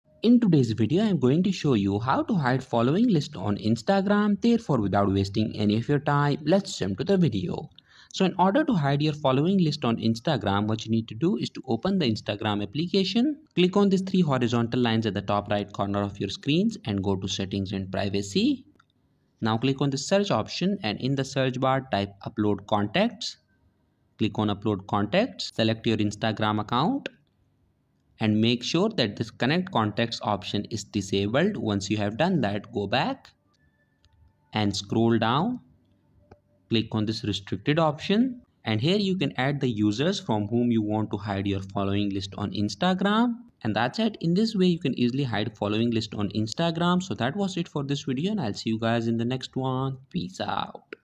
How to Hide Following on Instagram (2023)
In today's video, I am going to show you how to hide following list on instagram. If you are looking to hide your following list on instagram then this video is for you. So sit back and learn how to hide instagram following list.